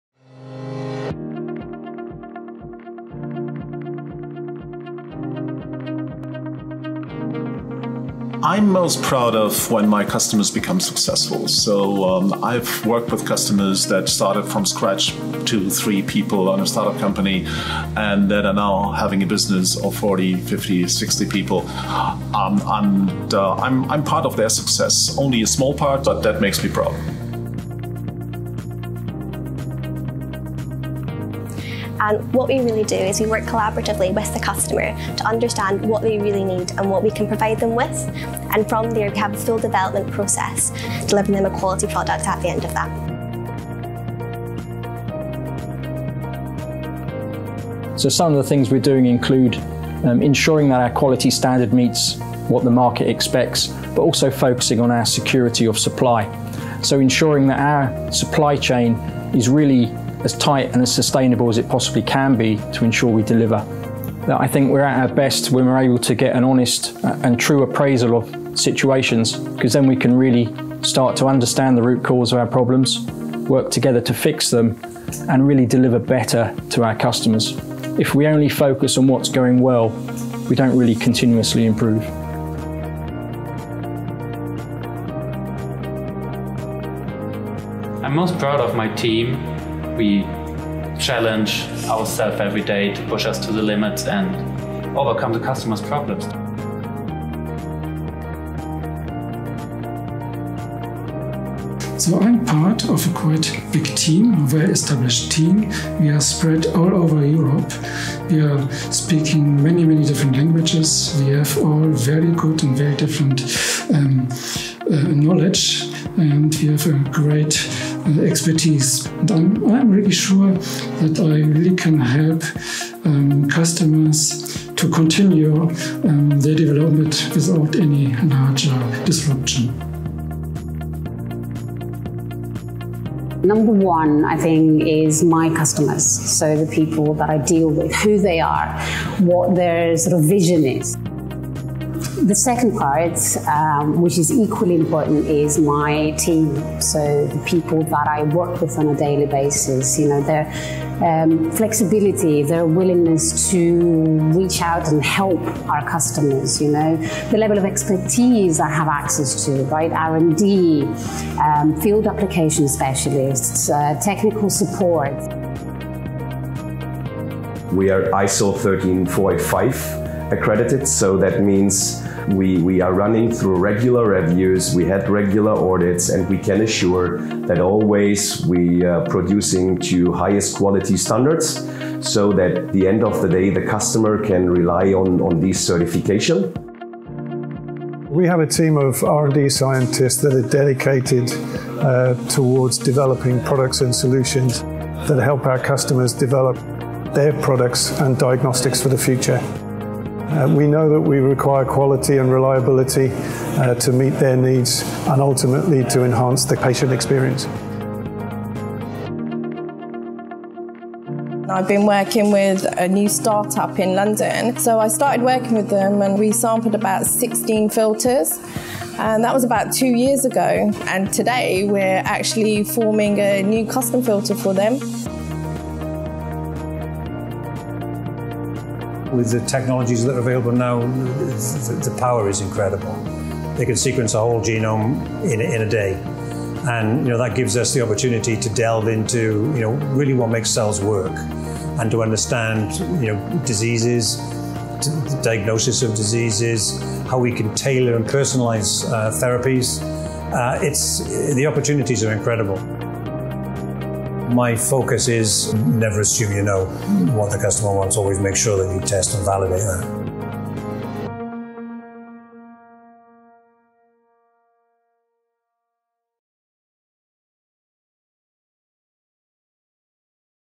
Advanced biopharma diagnostic solutions and technical support
Discover our advanced diagnostics solutions, tailored to meet the evolving needs of the biopharma diagnostics industry. In this video, our diagnostics team highlights a comprehensive range of offerings, from off-the-shelf products to fully customized solutions, designed to support diagnostic assay development and lateral flow diagnostics.

We combinee innovative technologies with expert technical support to help researchers and manufacturers develop, optimize, and scale their lateral flow services and diagnostic solutions. With capabilities ranging from product customization to troubleshooting and optimization, we deliver the tools and expertise needed to advance diagnostic applications.

What you’ll learn in this video:

An overview of advanced diagnostic solutions, including lateral flow diagnostics.
How we support diagnostic assay development with technical expertise and tailored services.
The flexibility and capabilities of our solutions, designed to accelerate diagnostic workflows.

Join us to learn how our advanced diagnostics solutions, combined with expert technical support, can help you achieve excellence in biopharma diagnostics and beyond.

Chapters:
00:00:00 Customer success stories and collaborative growth
00:00:41 Enhancing product quality and supply chain sustainability
00:01:15 Continuous improvement and team pride
00:02:13 Understanding our customers
00:02:54 Ensuring quality standards in product development
00:03:34 Advances in genome sequencing and personalized therapies
00:04:13 Importance of testing and validating customer needs